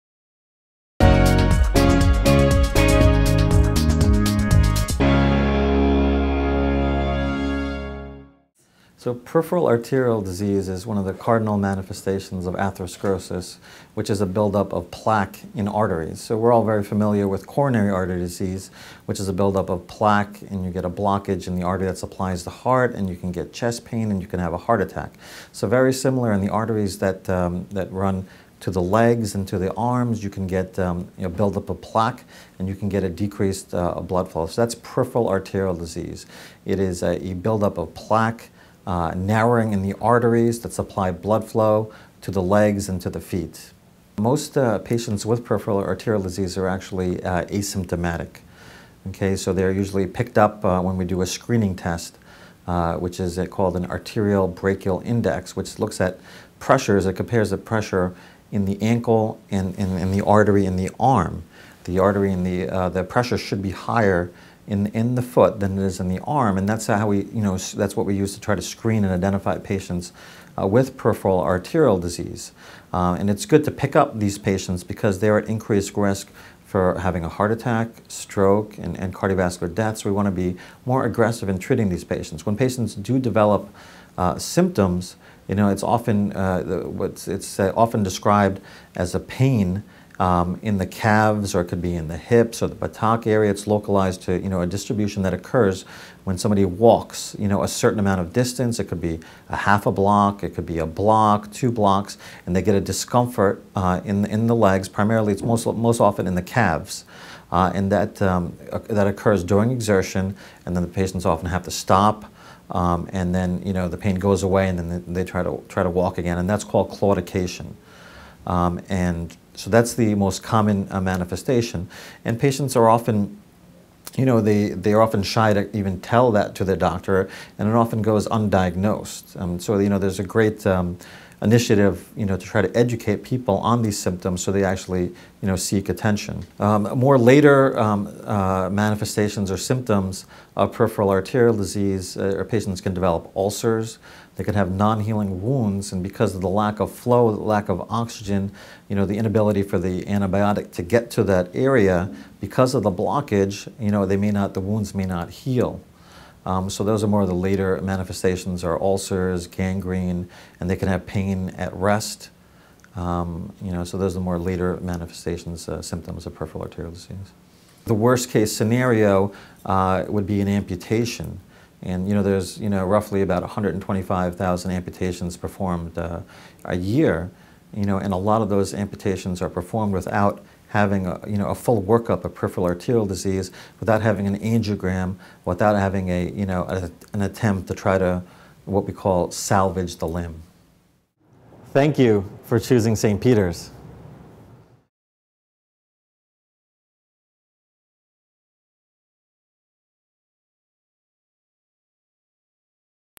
What is Peripheral Arterial Disease (PAD) and who is at risk? | Ask Saint Peter's
Ramzan Zakir, MD, director of the Peripheral Vascular Program at Saint Peter's, explains what peripheral arterial disease is - the build up of plaque in the arteries narrowing blood flow.  Find out who is at risk.